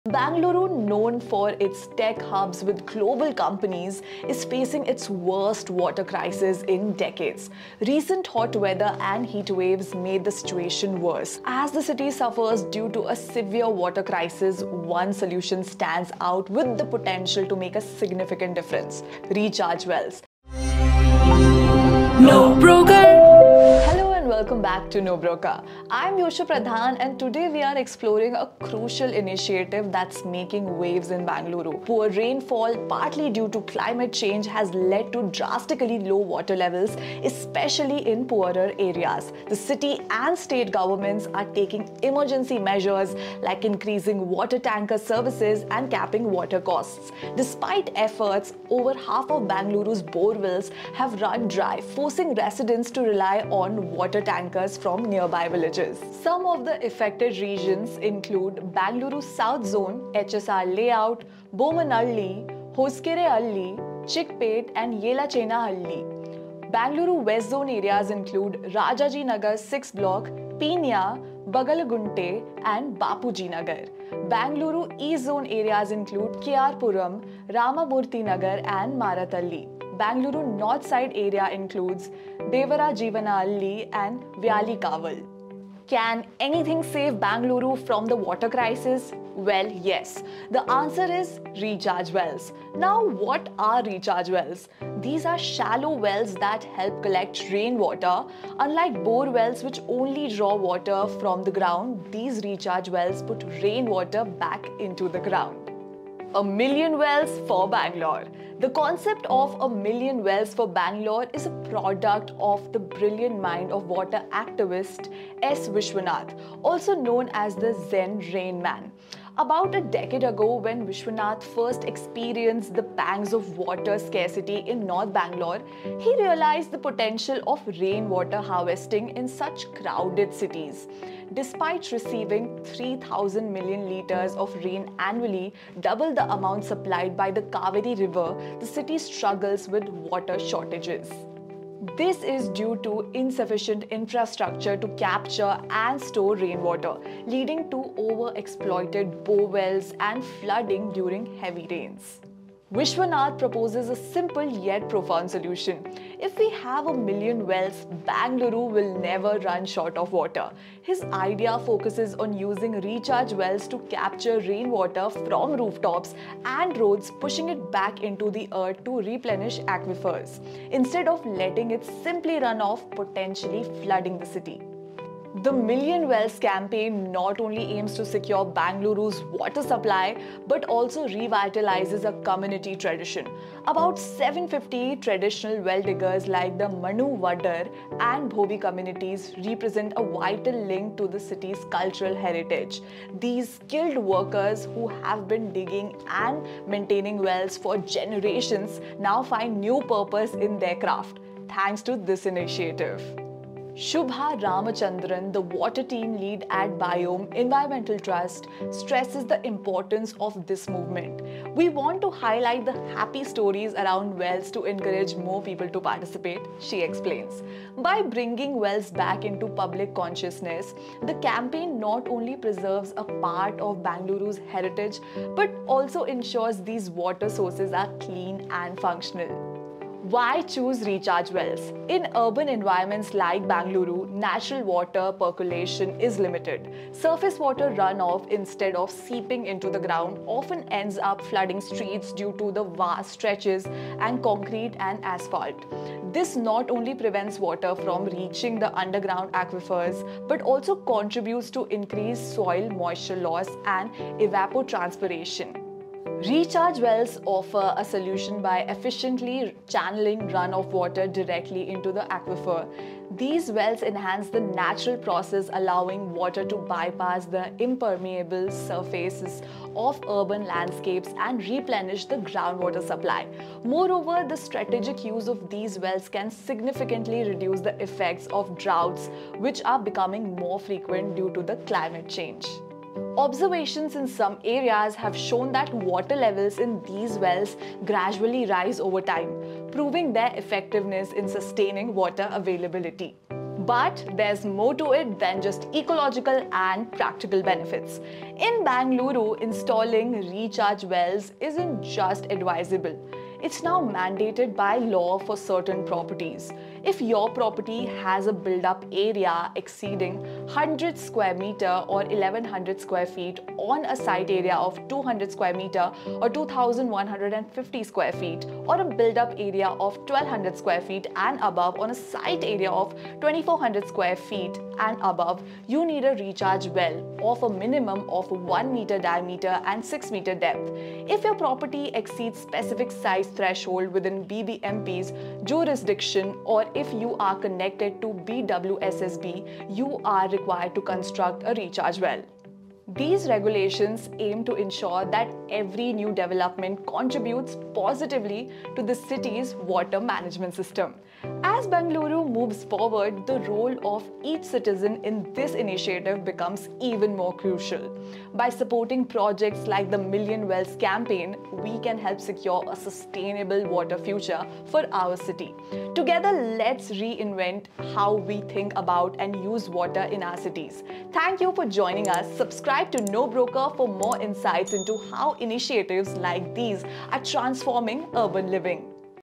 Recharge Wells - Opt-in Solutions for Bangalore Water Crisis
🚱💧 Welcome to NoBroker! Join us as we explore the critical water crisis in Bengaluru and the innovative solution of recharge wells that promises to transform urban water management. With the city grappling with severe shortages, exacerbated by heatwaves and poor rainfall, we delve into how recharge wells can help replenish groundwater and ensure sustainability.

*🕒 Timestamps:*
00:00-00:23 – Introduction: Understanding Bengaluru's Water Crisis
00:24-01:25 - Examining the Most Affected Regions in Bengaluru
01:26-01:46- Explaining Recharge Wells: A Sustainable Solution
01:47-03:49 – A Million Wells for Bangalore
03:49-04:53 - The Urban Impact: Benefits of Recharge Wells
04:54-05:51 – Legal Obligations for Installing Recharge Wells in Bengaluru
05:52-06:19 - Conclusion

Overview of Bengaluru's Water Crisis: Impact of climate change and urban development.
Exploration of Recharge Wells: How these structures can capture rainwater to fight scarcity.
A Million Wells for Bangalore: Initiatives by water activist S Vishwanath and their potential to change the city's future.
Benefits and Legal Mandates: Understand the advantages and city regulations surrounding the installation of recharge wells.

Stay informed about the innovative measures transforming cities like Bengaluru into sustainable urban spaces.

*Download NoBroker Apps*
*Android*